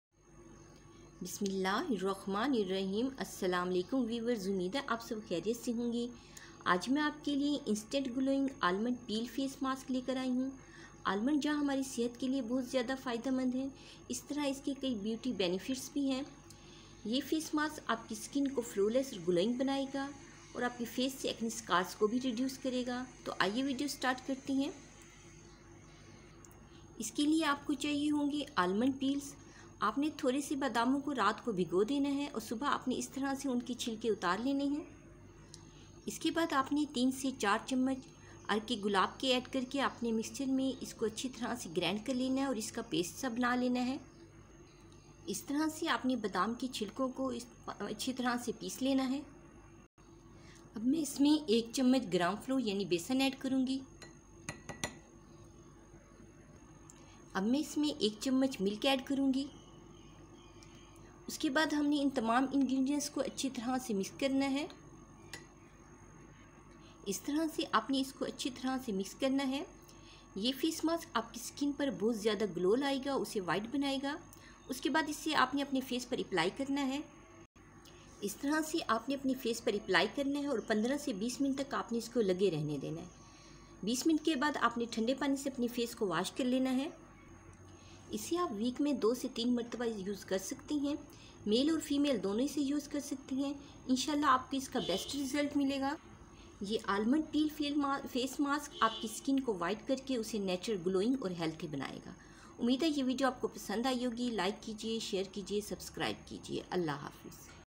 How to Get Even Skin Tone Naturally uses of Almond Peel Face mask
in this video Iam sharing you natural home remedy for glowing Fair Clear Skin almond one of the best ingredients for skin whitening and beautiful this almond face mask give you healthy and beautiful skin your skin looks like a even tone and reduce dark spots,are you like my videos subscribe My channel#beautytips#skincare#fairskin#jamilabano